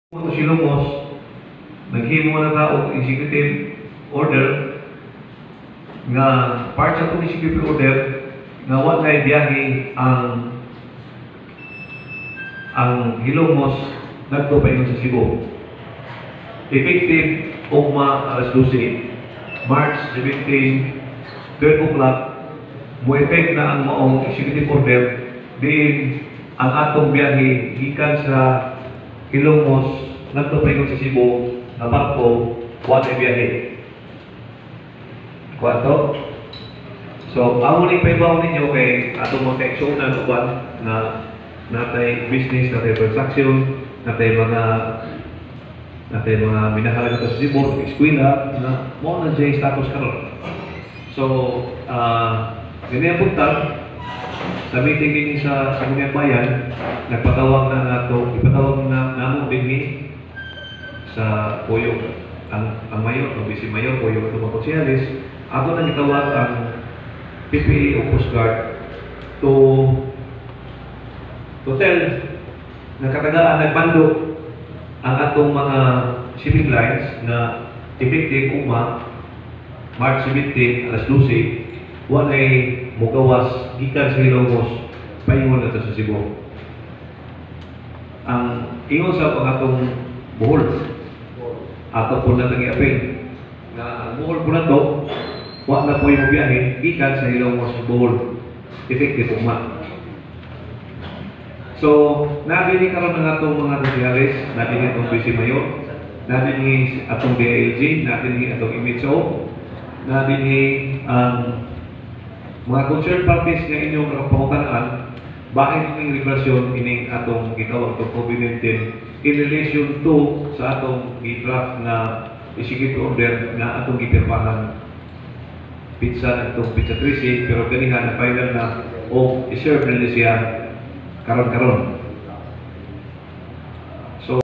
Hilongos Mayor Albert R. Villahermosa on his message about Coronavirus.
March 16, 2020 at Sangguniang Bayan Session Hall, Legislative Building, Municipalility of Hilongos, Leyte.